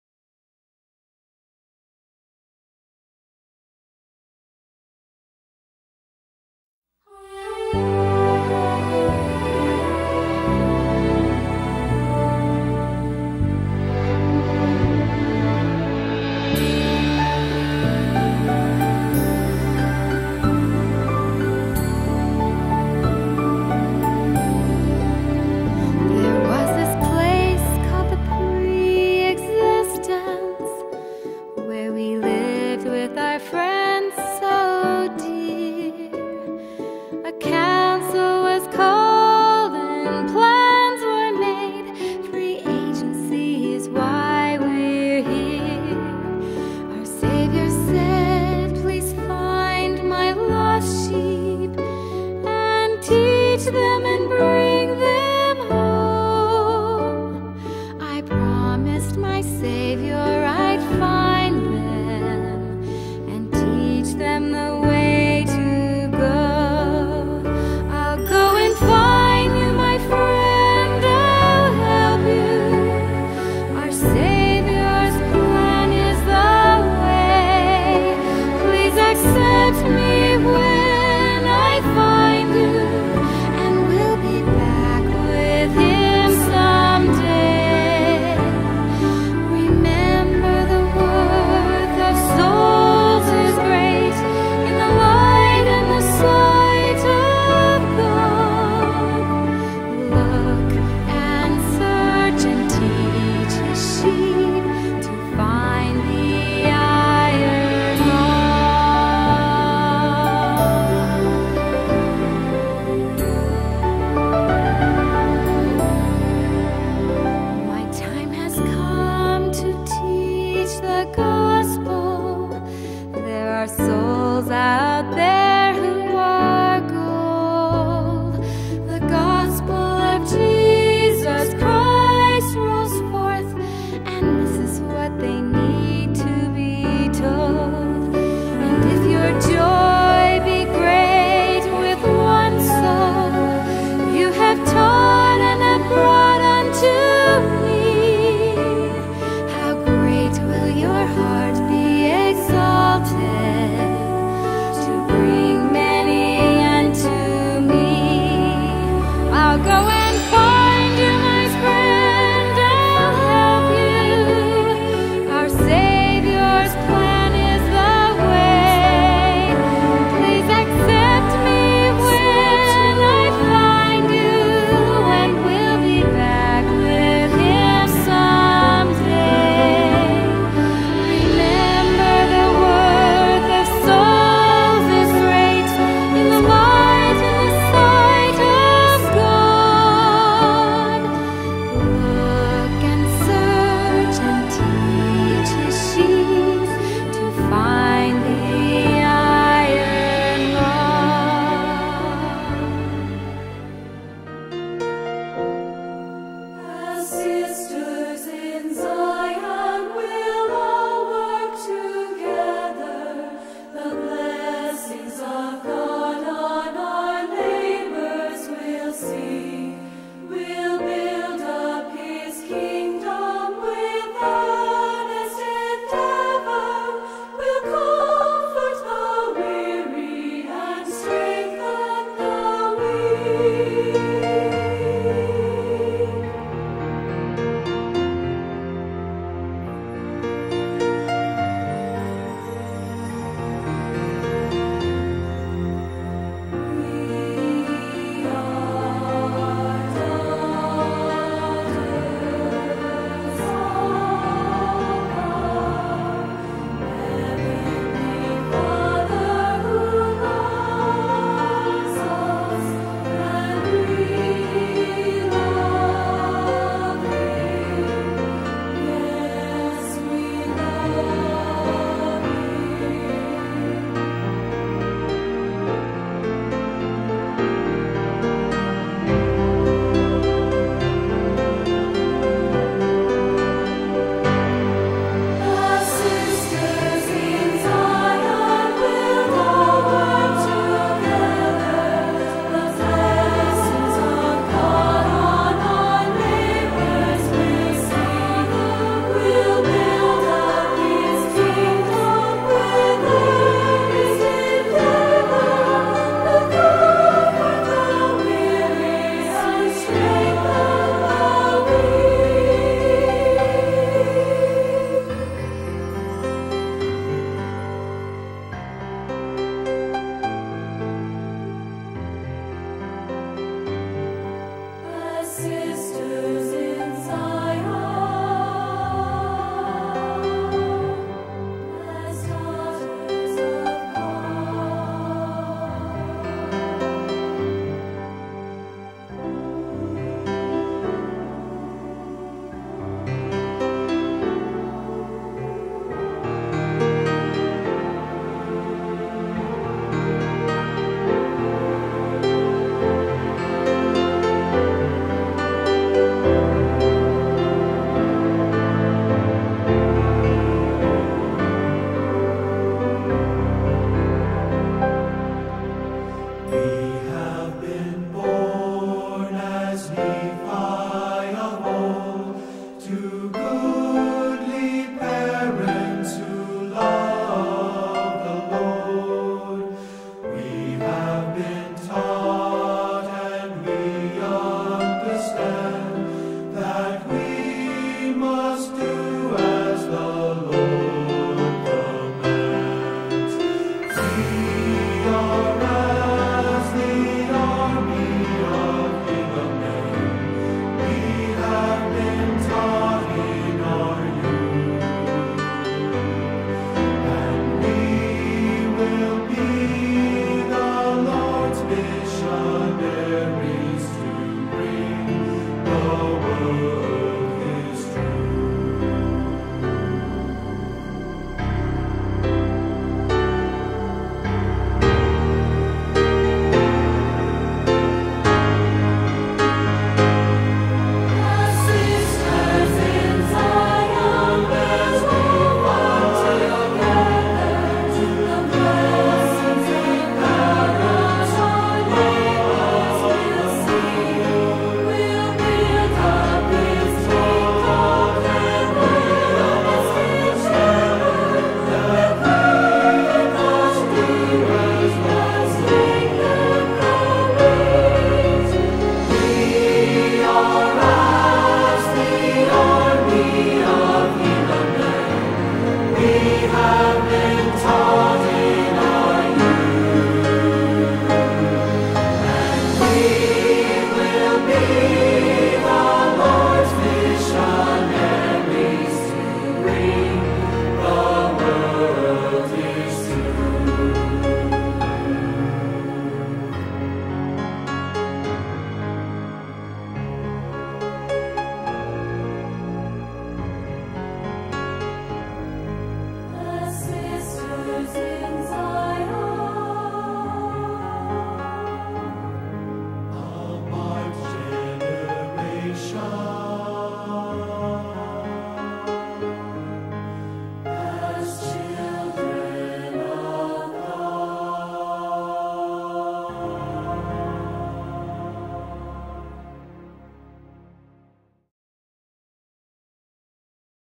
Aoyagi Talk Tokyo Reunion August 1, 2009
Elder Aoyagi's talk at Sendai Mission reunion in Tokyo on August 1, 2009